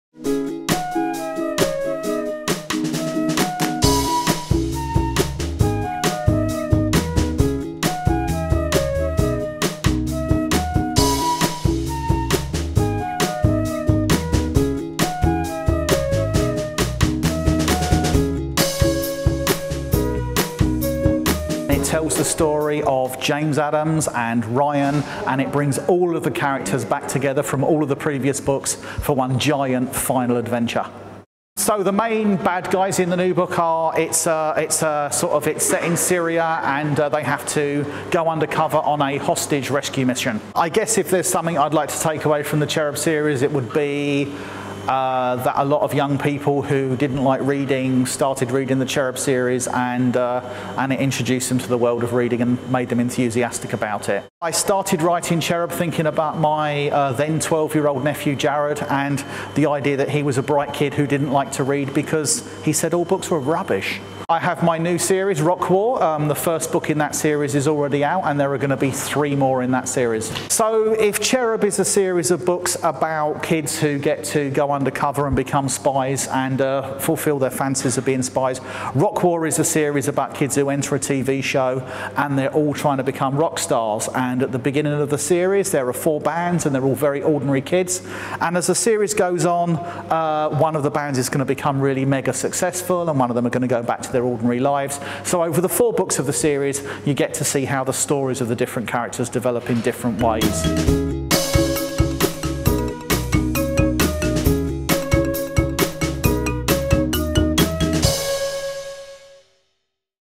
Robert Muchamore - Cherub. Volume 17, Commando Adams
A l’occasion du salon du livre et de la presse jeunesse à Montreuil, rencontre avec Robert Muchamore auteur de l'ouvrage «Cherub. Volume 17, Commando Adams» aux éditions Casterman.